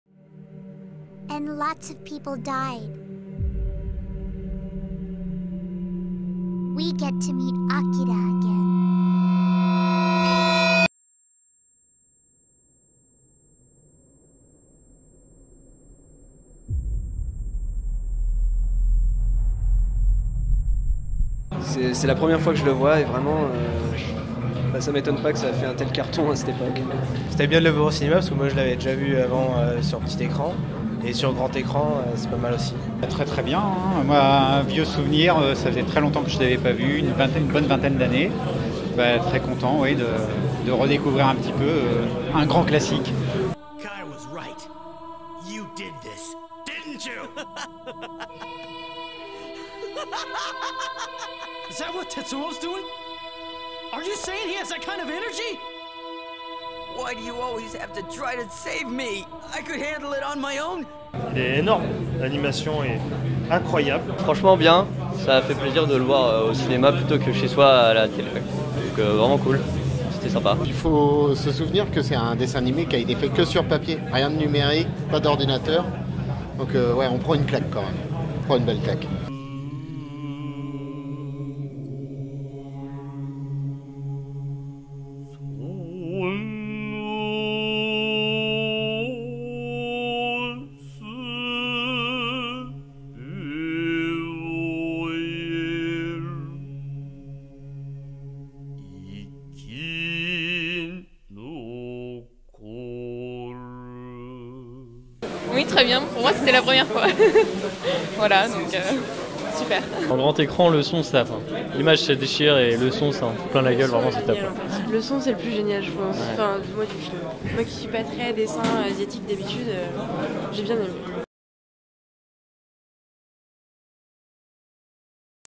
Les Enfants de l'Absurde - Sortie de Séance "Akira"
Les avis des spectateurs de l'absurde Séance à la sortie du film "Akira" de Katshiro OTOMO.